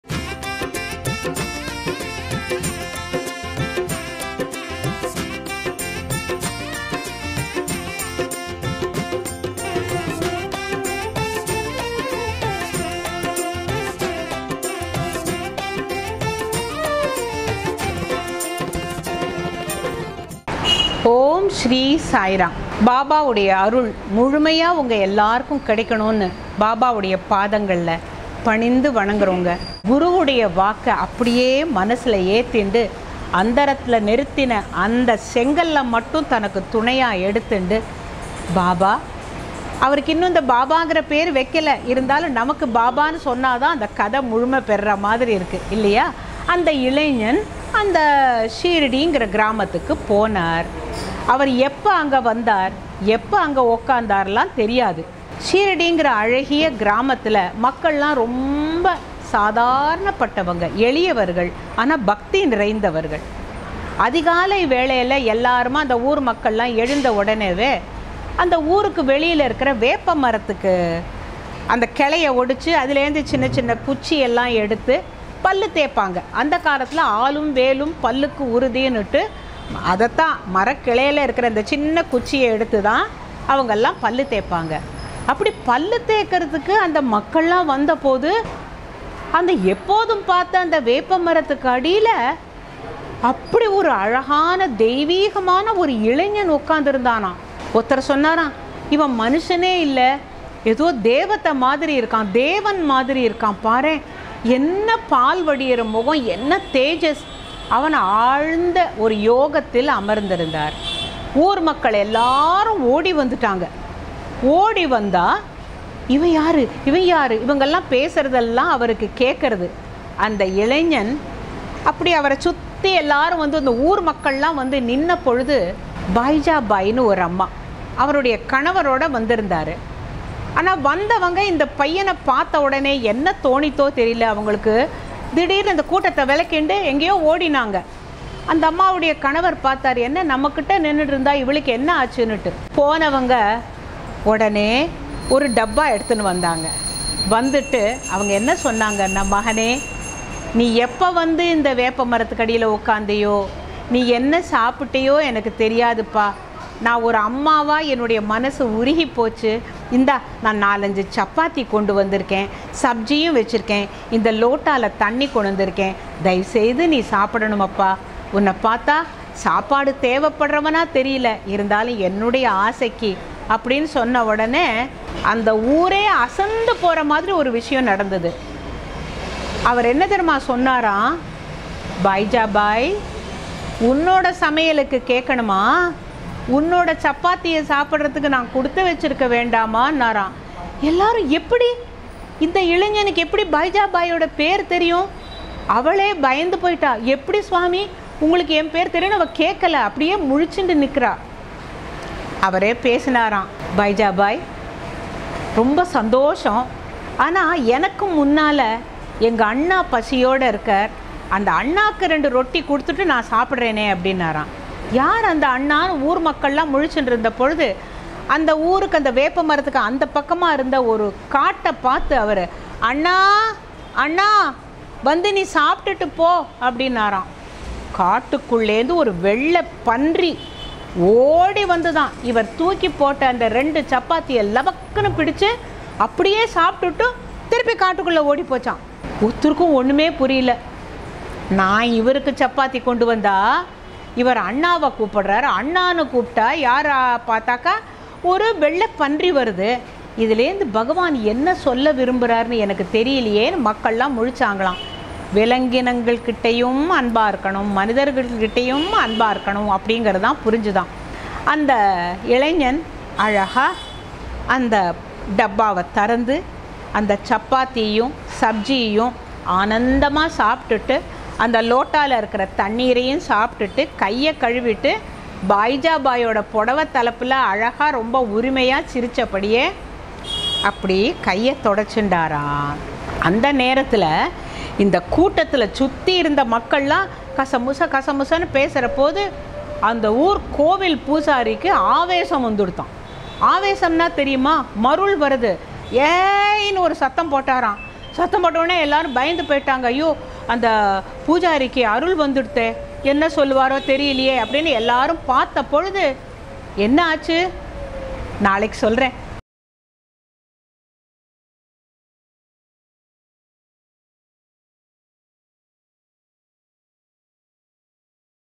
Life of Sai Baba - Part 04 | சாய் சத்சரிதம் 04
Kalaimamani Smt. Revathi Sankaran narrates the life of Shirdi Saibaba.